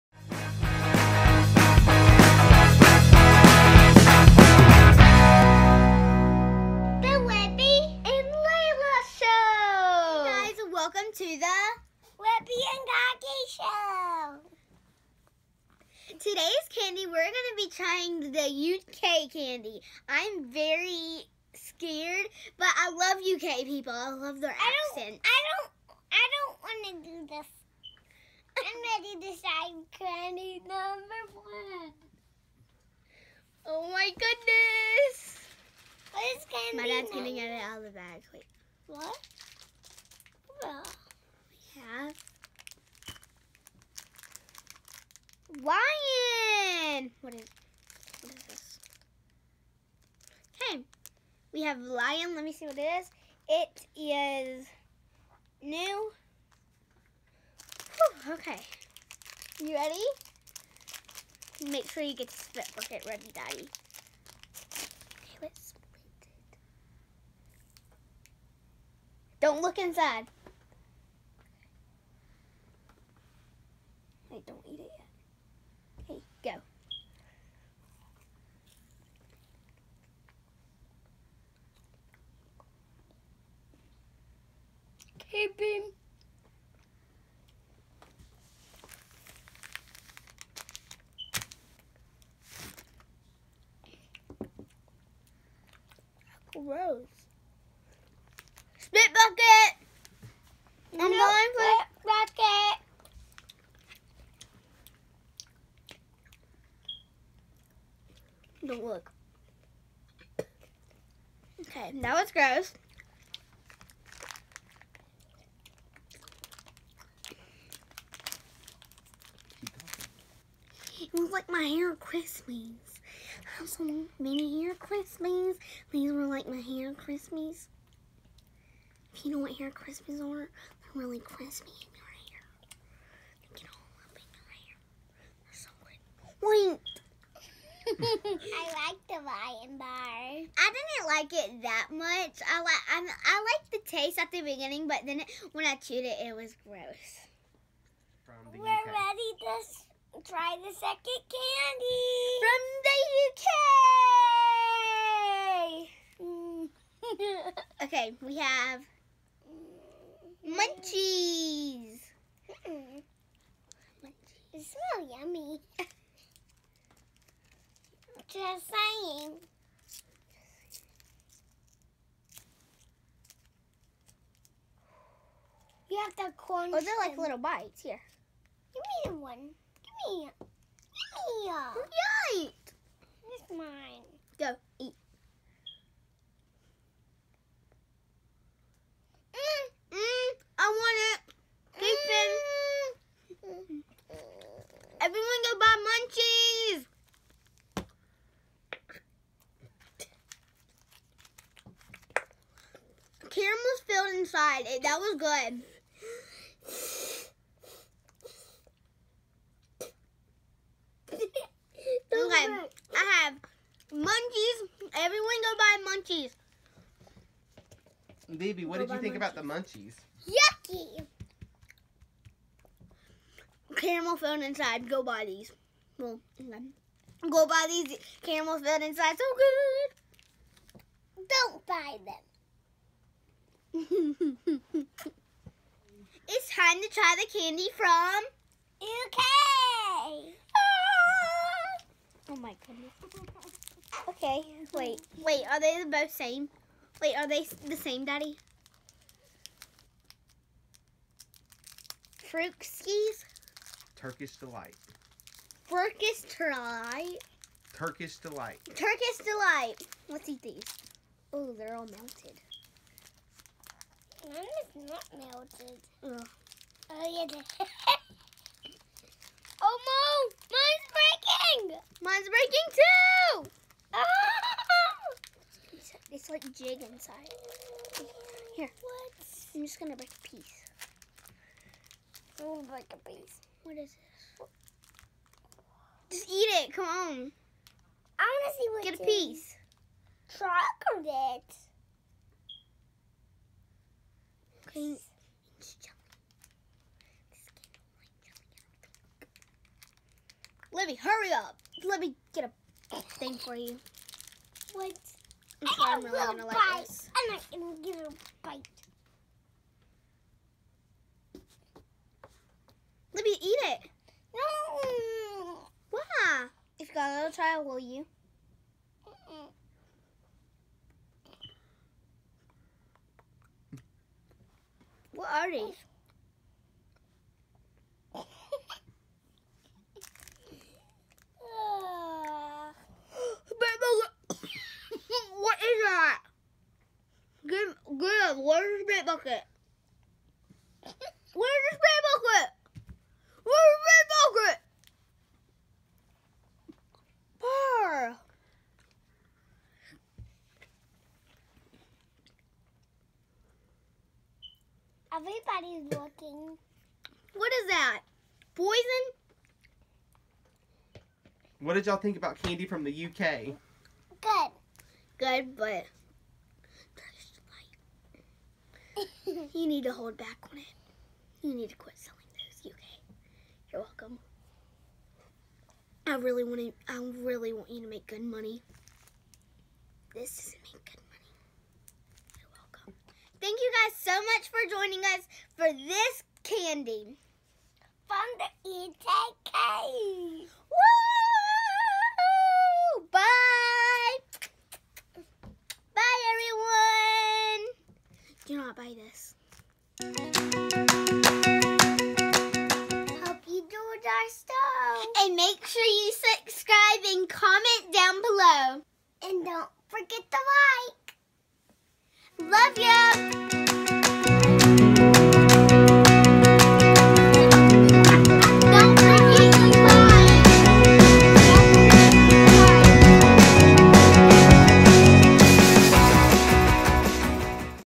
Candy Around the World (UK) - Libby & Layla Show Episode 3
Come join Libby & Layla on their journey as they try candy from around the world!  In this episode, they are trying candy from United Kingdom!

Come join us on all social media!
Search LuLuGaGa09 on...

Boost Layla on Famous Birthdays!